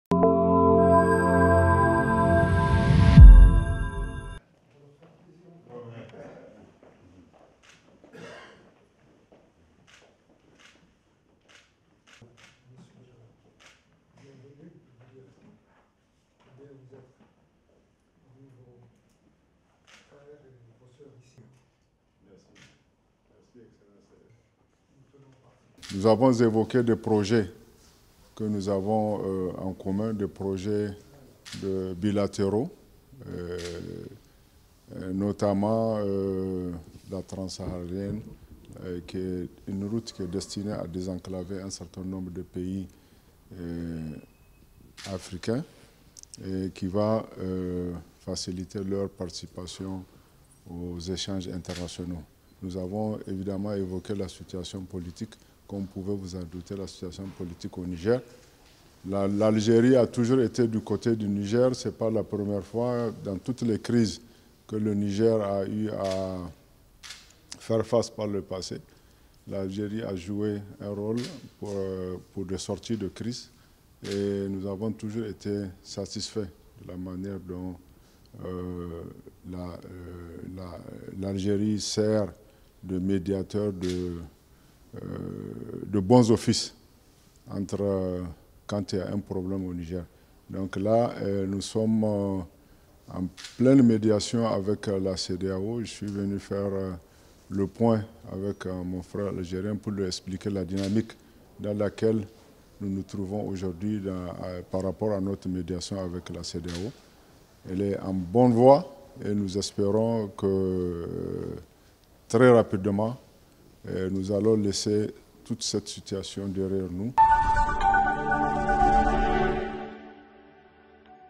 عطاف يستقبل وزير الشؤون الخارجية والتعاون والنيجريين بالخارج
الجزائر (واج) - استقبل وزير الشؤون الخارجية والجالية الوطنية بالخارج, السيد أحمد عطاف, اليوم الثلاثاء, بمقر الوزارة, وزير الشؤون الخارجية والتعاون والنيجريين بالخارج, السيد باكاري ياوو سانغاري, الذي حل
اليوم بالجزائر في إطار زيارة عمل.